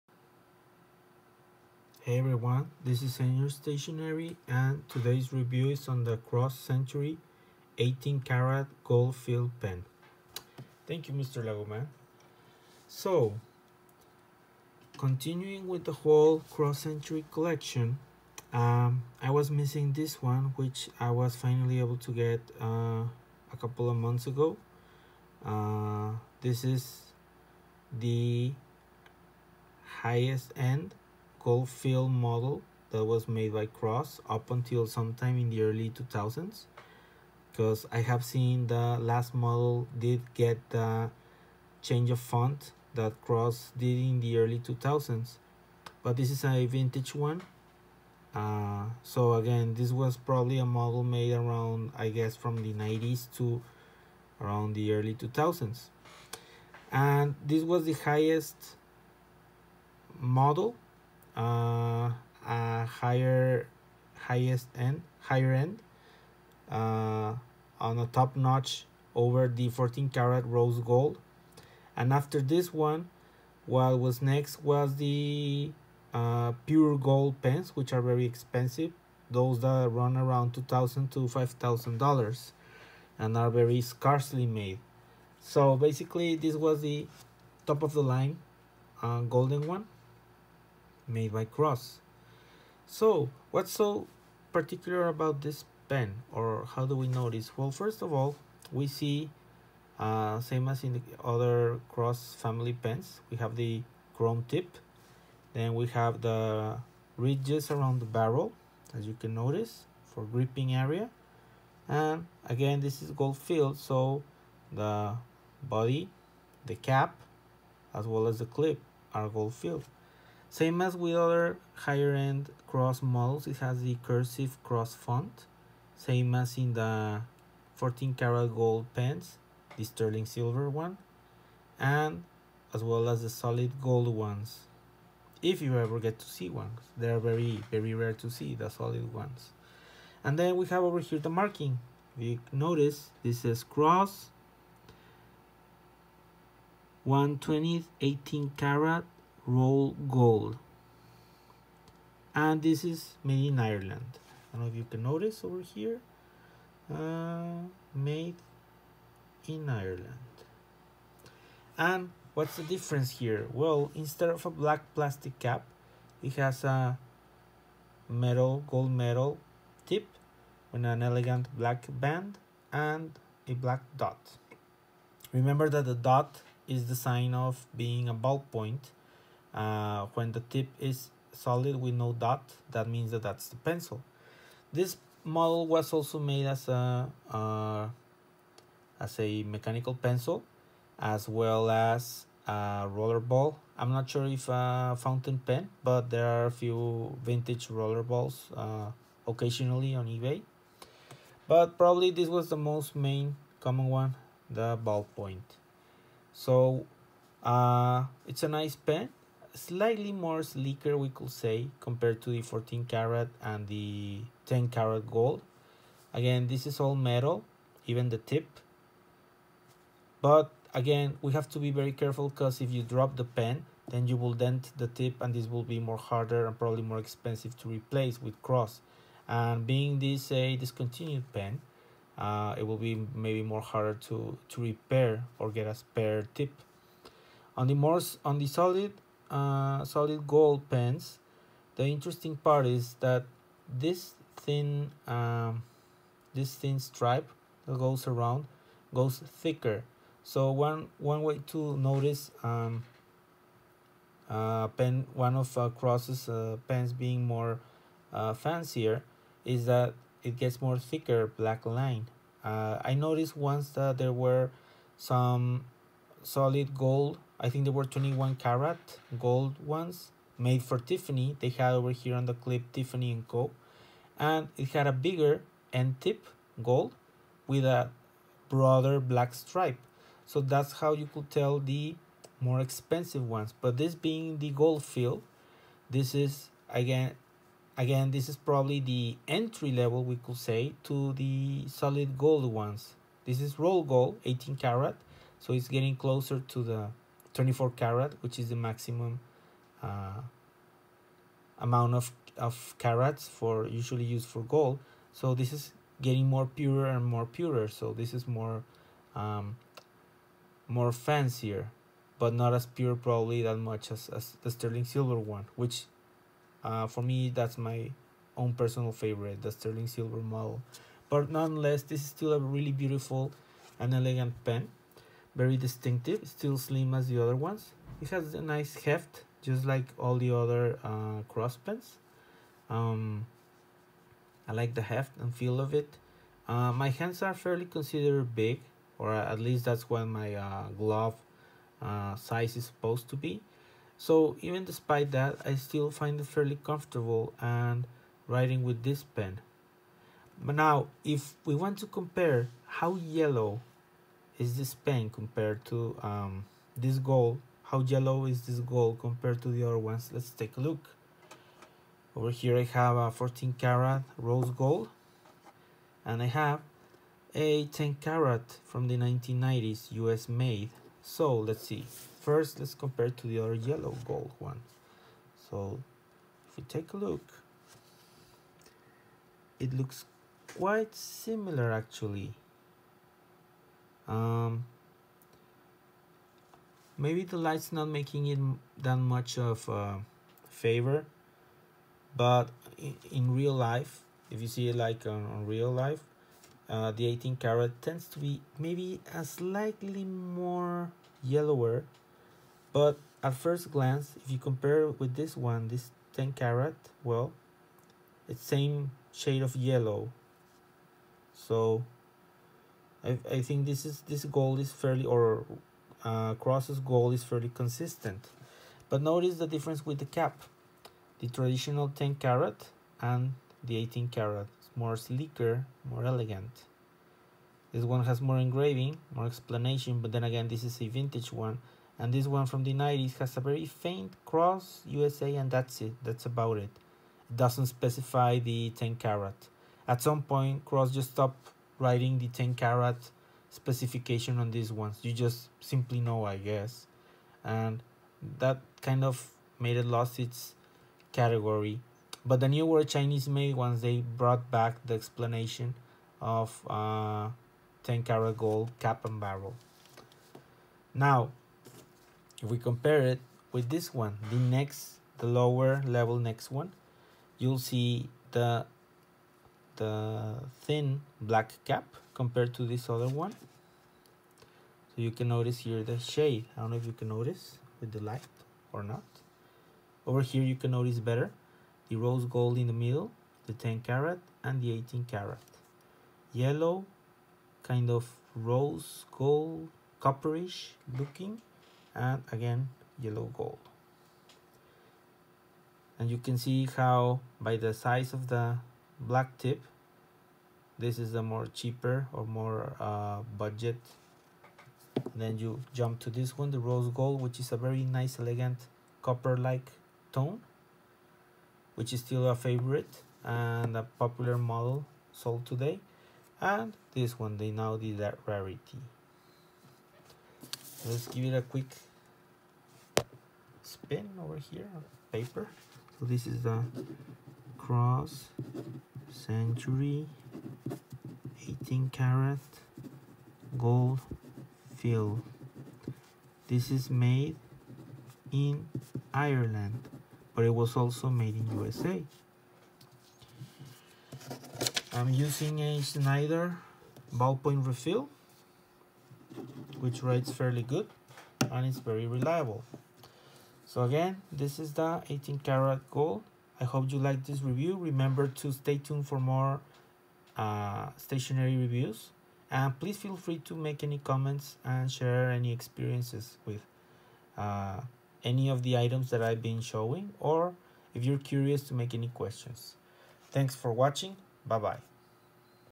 Cross Century 18KT Gold Filled Ballpoint pen Review
My review on the Cross Century 18KT Gold Filled model. We take a look at this now rare discontinued model which was the high end version before the solid gold Century models still sold today. The 18KT Gold Filled was produced until the early 2000's as there are some with the new Cross font. This one is an earlier version with the vintage Cross font printed on cap and cursive font on pocket clip. If you like gold or gold color, this is the pen for you!
It was made in USA or Ireland and it has a gold metal end tip with black band on mid area compared to the well known 10KT and 14KT which have black plastic end tips. If you are not sure if you want the solid gold versions of 18KT or 21KT, then this model could fill that spot in your collection.

This model was also made in Rollerball and Fountain Pen up until 1990s and today only made in ballpoint and pencil.

Keep writing! :)